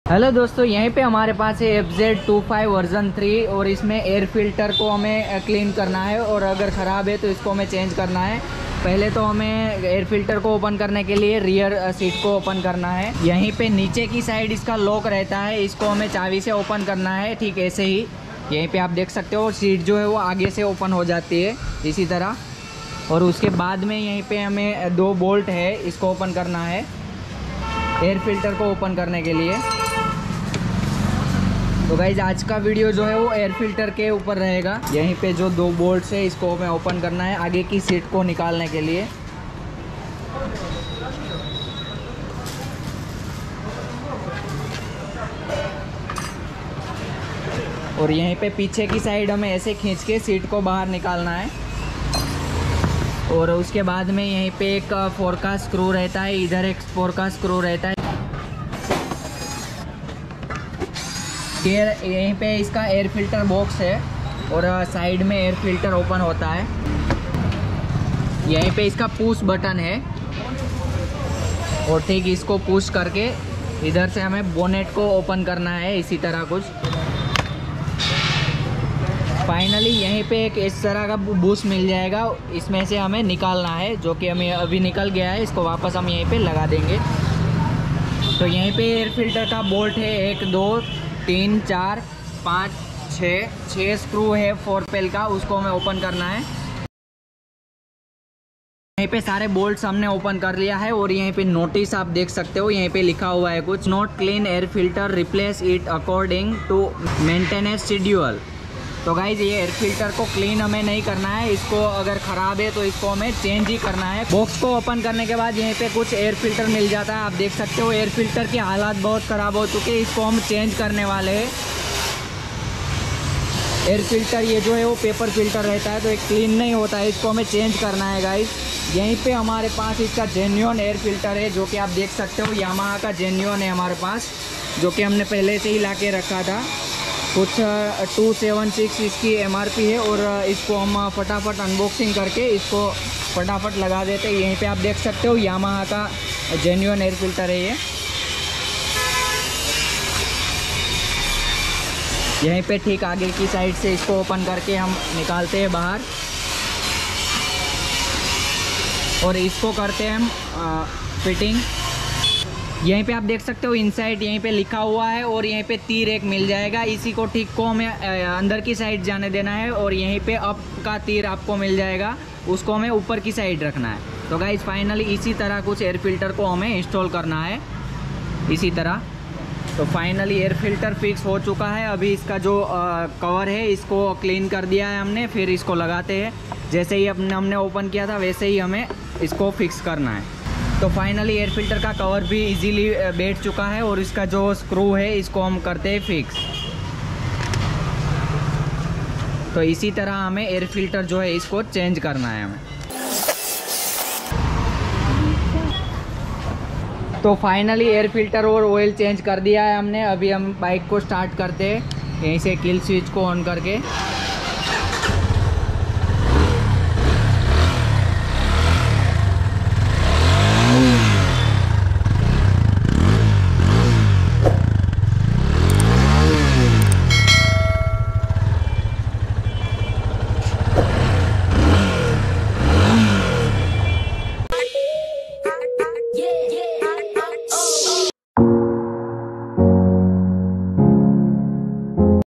How to Replacement Air Filter in Yamaha FZ and all Bike🏍️ | Bike pickup problem | Hindi
FZ25 V3 Air filter change Shreenathji auto Parts and Garage 👍

@NCSMusic us in this video